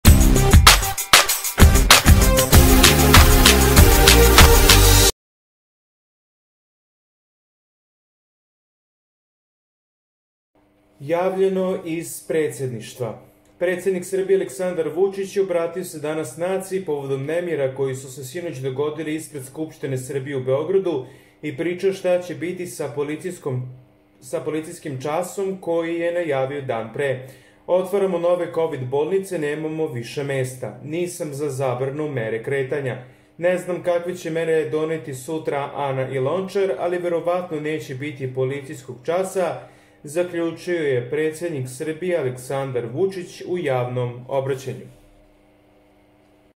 Vucic se HITNO obratio naciji: Nece biti policijskog casa
Javljeno iz Predsedništva.